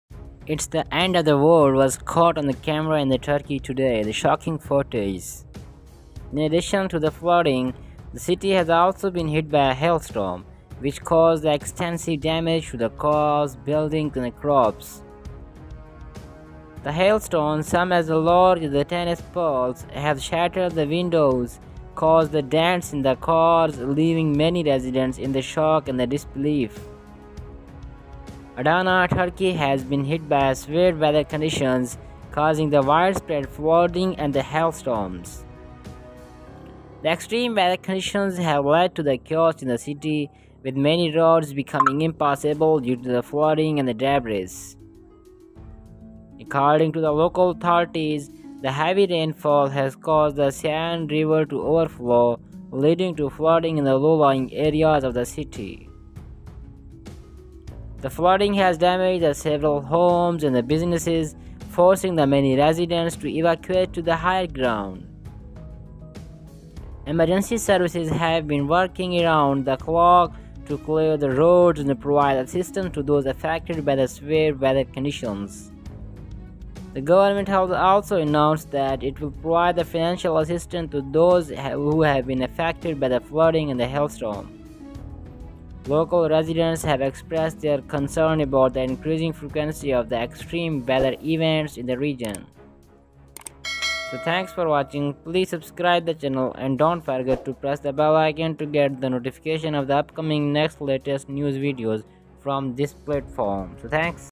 The end of the world was caught on camera in Turkey today! Shocking footage!
In addition to the flooding, the city has also been hit by a hailstorm, which has caused extensive damage to cars, buildings, and crops. The hailstones, some as large as tennis balls, have shattered windows and caused dents in cars, leaving many residents in shock and disbelief.

Adana, Turkey, has been hit by severe weather conditions, causing widespread flooding and hailstorms. The extreme weather conditions have led to chaos in the city, with many roads becoming impassable due to flooding and debris.

According to local authorities, the heavy rainfall has caused the Seyhan River to overflow, leading to flooding in low-lying areas of the city. The flooding has damaged several homes and businesses, forcing many residents to evacuate to higher ground.

Emergency services have been working around the clock to clear the roads and provide assistance to those affected by the severe weather conditions. The government has also announced that it will provide financial assistance to those who have been affected by the flooding and hailstorm.

Local residents have expressed their concern about the increasing frequency of extreme weather events in the region. Many are calling for better infrastructure and preparedness measures to be put in place to mitigate the impact of future weather events.